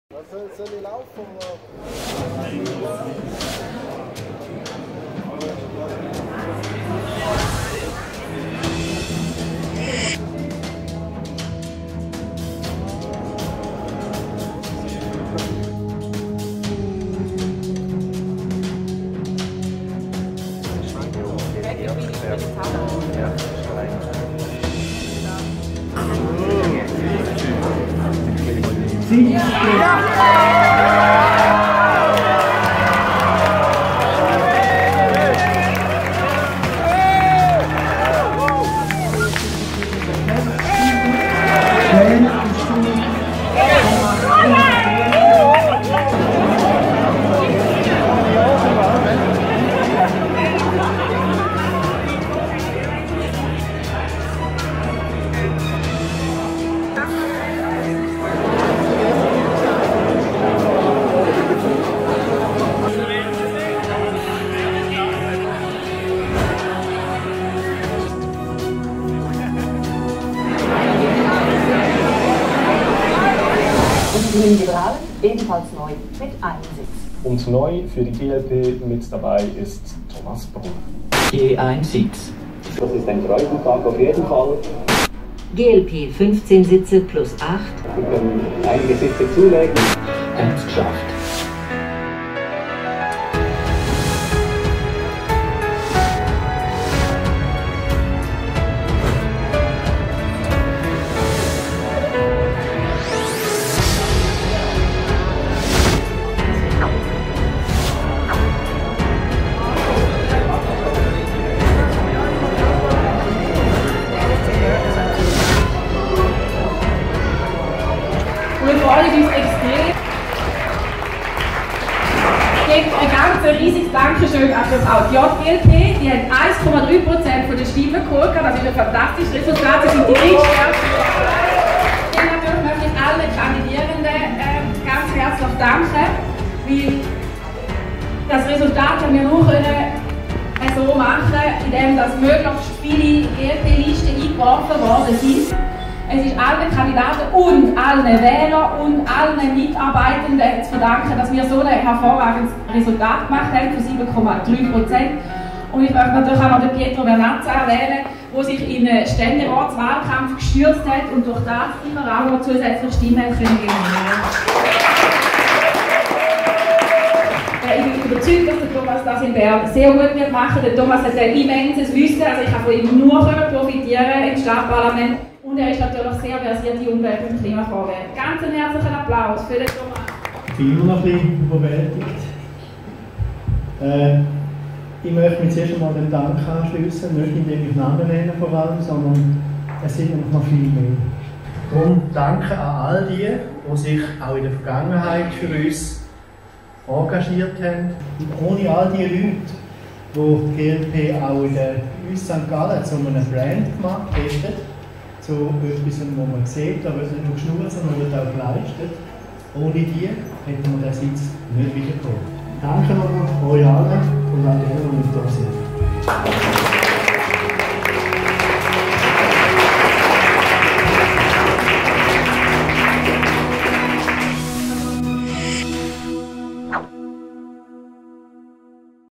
Wahltag in St.Gallen, 20.10.2019 – glp erobert einen Sitz im Nationalrat
Spannung, Jubel, Feier – Thomas Brunner wird zum Nationalrat gewählt.